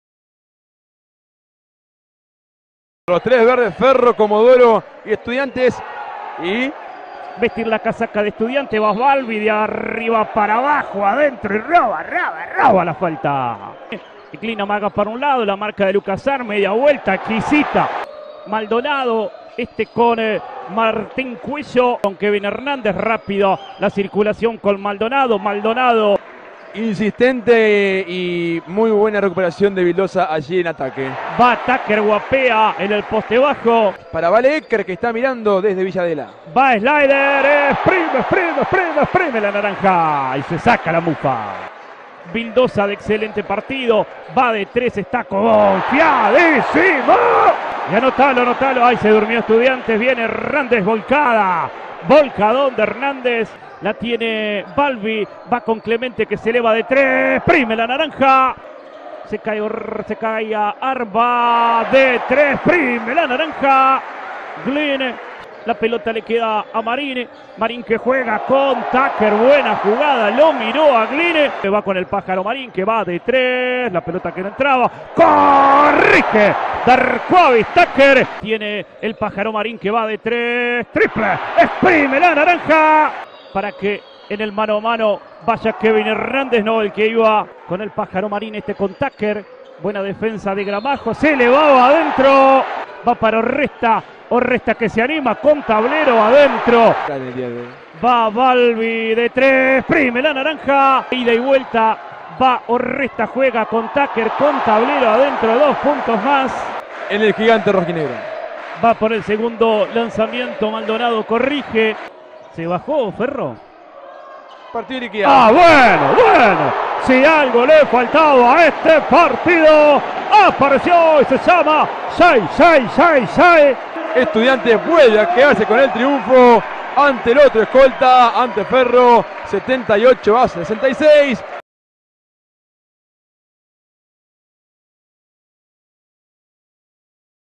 23.04.2017 | Estudiantes (C) vs. Ferro
Triunfo de Estudiantes (78-66), en condición de local frente a Ferro, en El Gigante Verde de Concordia.